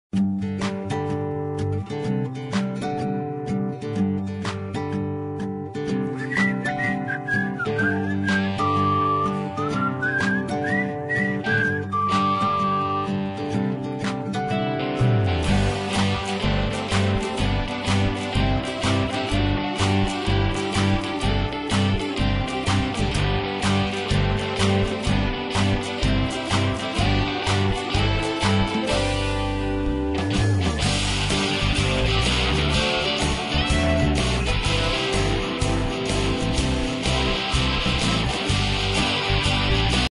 Конверт зимний в этническом стиле
Красивый конверт с меховой подкладкой - зимний вариант для выписки мальчика!